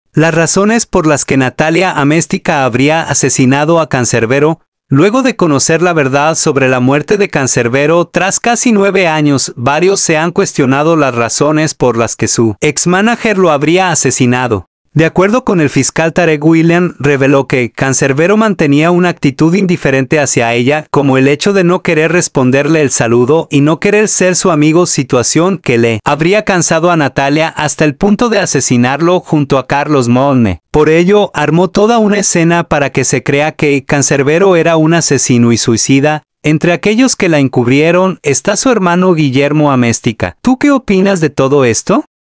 CANSERBERO: Las razones por las que NATALIA AMÉSTICA le HABRÍA quitado la vida
LA RAZON QUE LLEVÓ A NATALIA AMÉSTICA ACABAR CON LA VDA DE CANSERBERO.  Sobre Carlos Molnar, esposo por más de una década de la representante, el fiscal denunció que estos se debieron a resentimientos por pleitos profesionales y económicos, los cuales la llevaron a acabar con su pareja y culpar al rapero del crimen.